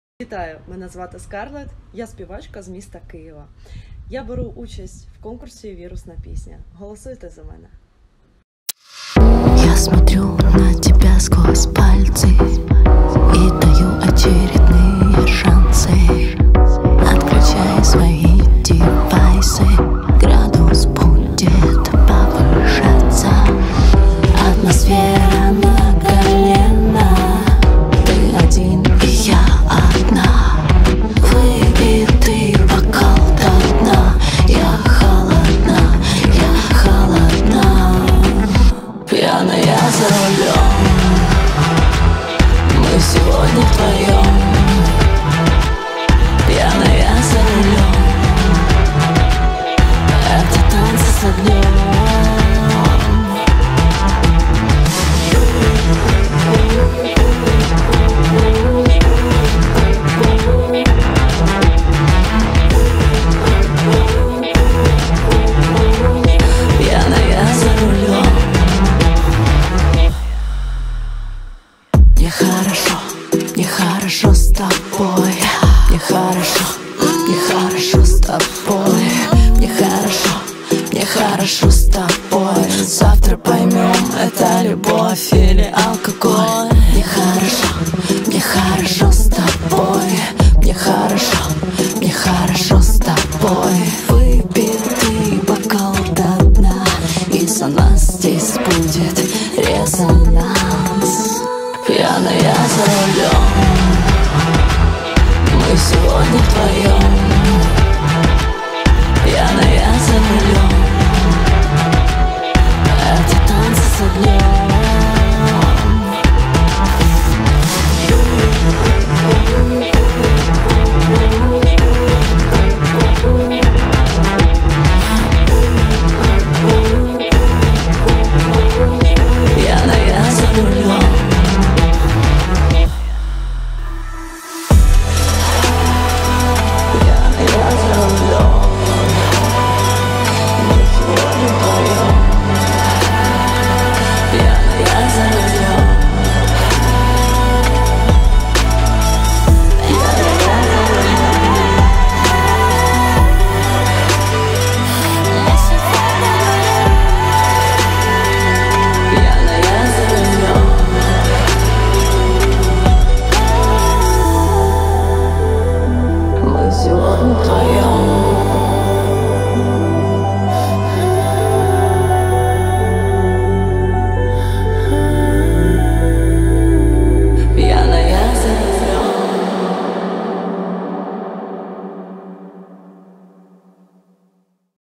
Евгения Скарлет Киев Пьяная за рулём Украинская певица,автор песен,финалистка конкурса Мисс Украина
Евгения Скарлет из Киева Пьяная за рулём  Украинская певица,автор песен,финалистка конкурса Мисс Украина-Участница Народного конкурса "Вирусная Песня".. В данный момент проживает в г Торревьеха Испания